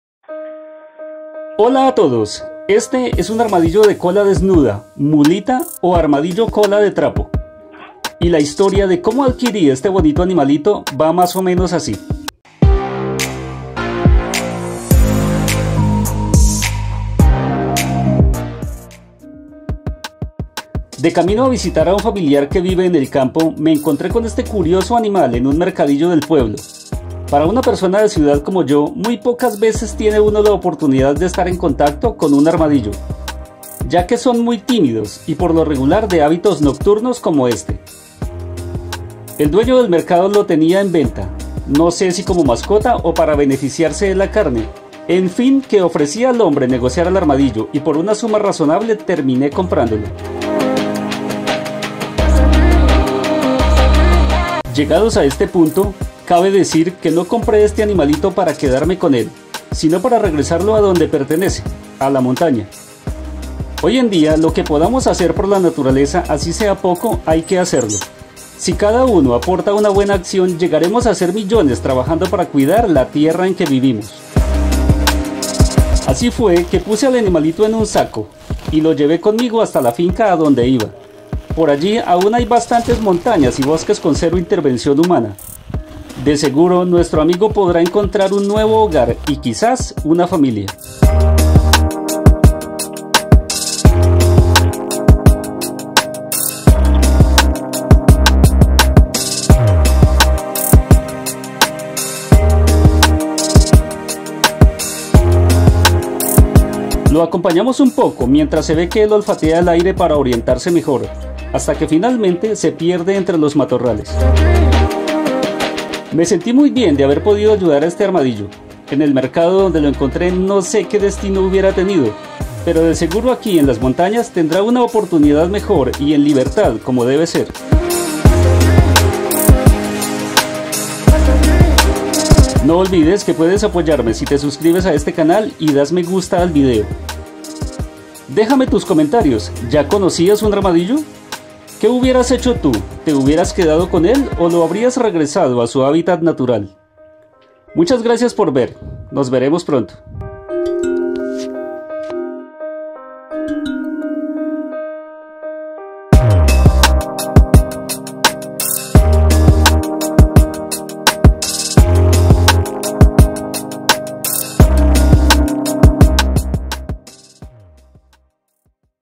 COMPRANDO UN ARMADILLO
Hola a todos, Este es un armadillo de cola desnuda, mulita o armadillo cola de trapo.
Y la historia de cómo adquirí este bonito  animalito va mas o menos asi.